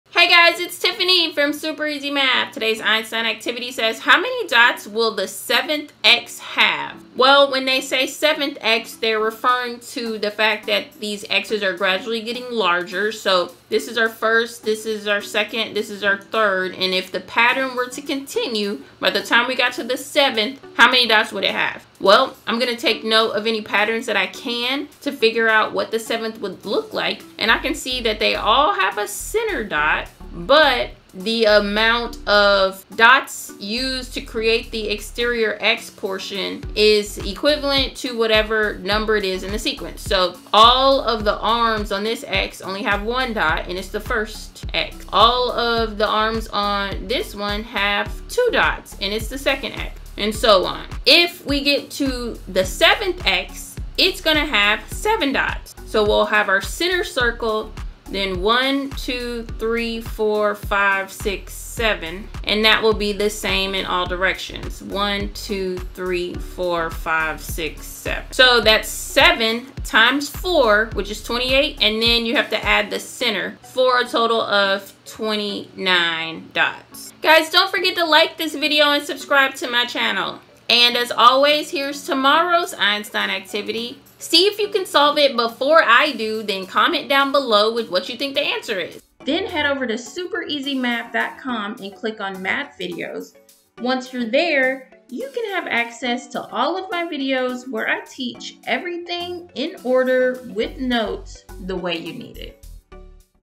How many dots will the 7th X have?
In this video, I'll show you how to solve this problem.

Get access to all of my videos where I teach everything in order with notes with this

Need some help?

Do you have more questions before you join? Complete this form and we will get in touch with you